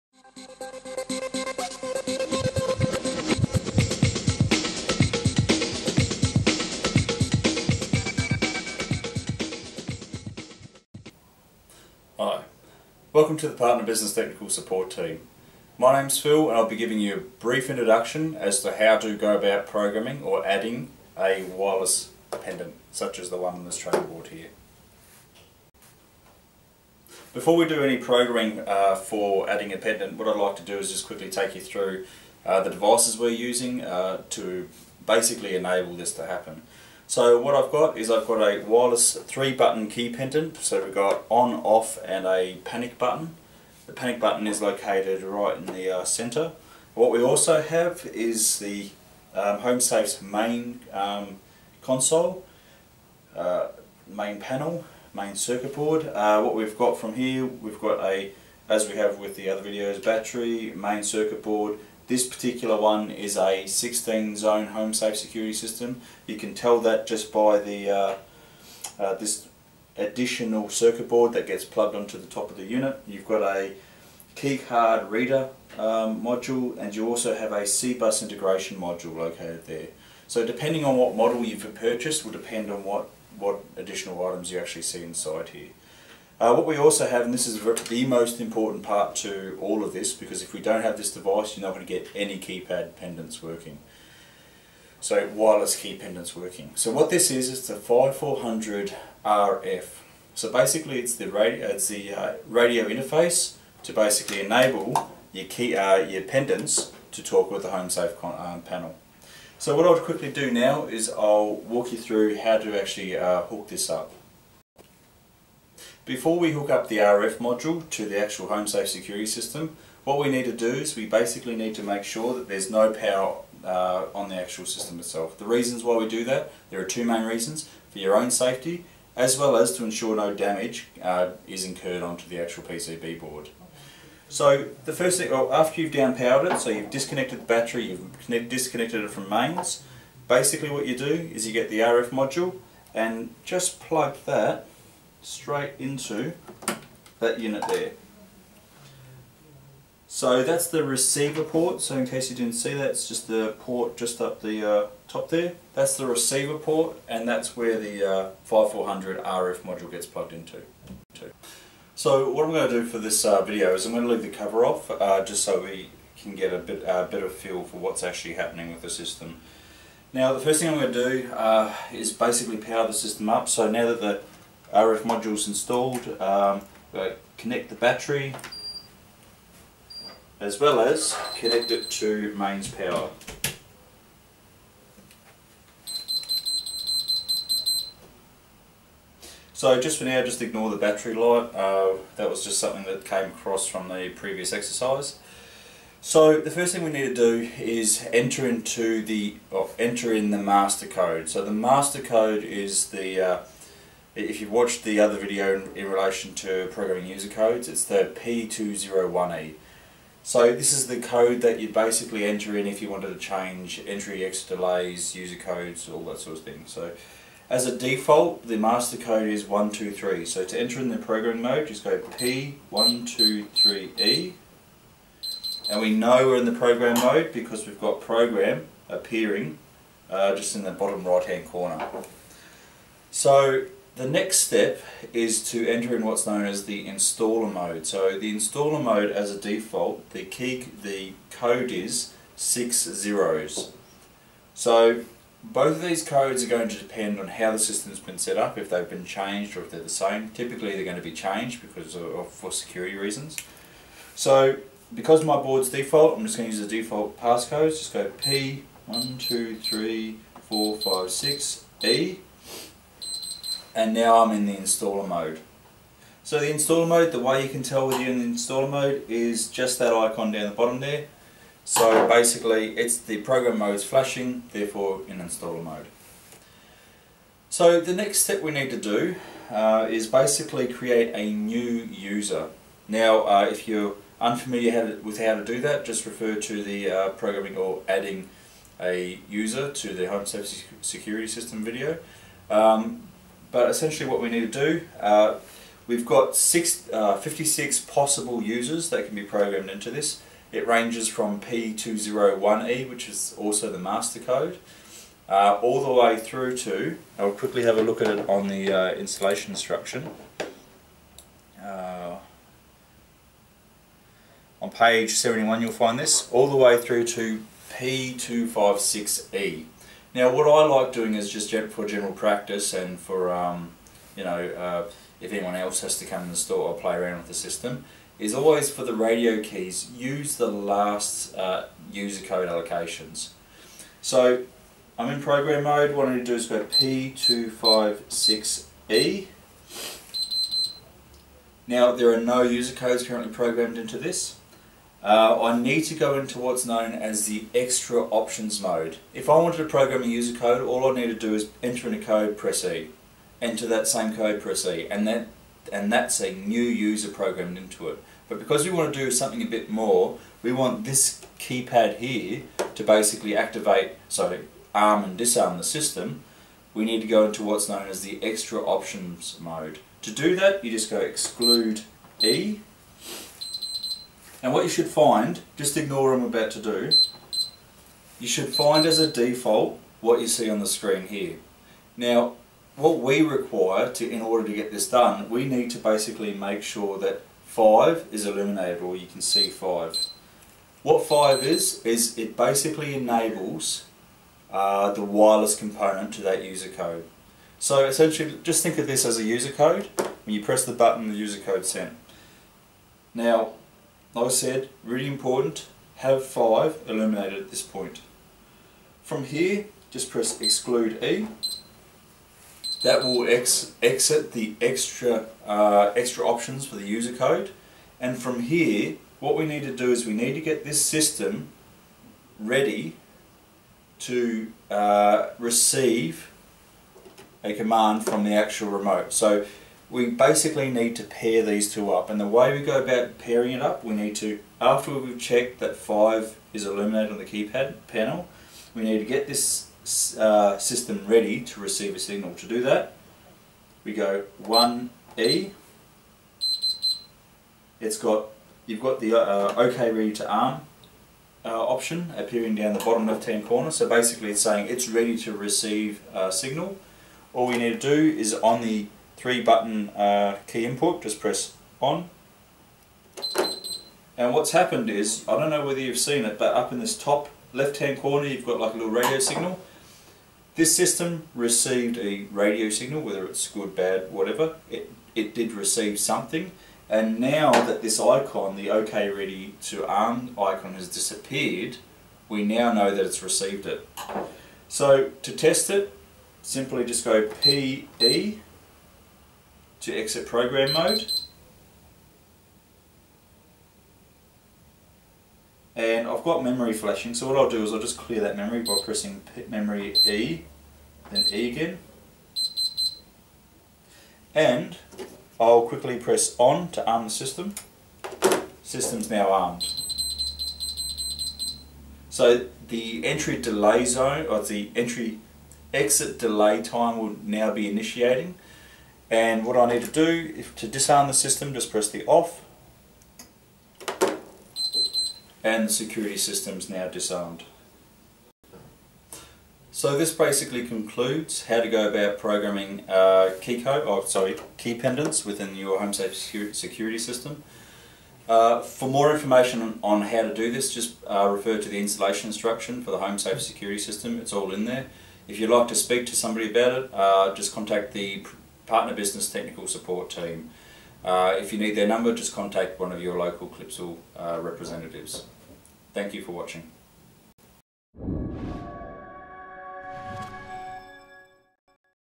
HomeSafe Adding A Wireless Pendant
WARNING: The following video is intended only for person(s) suitability trained, qualified and licenced to undertake such a task.

The following video provides a quick guide in how to add a wireless pendant.

If you have found this video useful, please let us know by pressing the like button.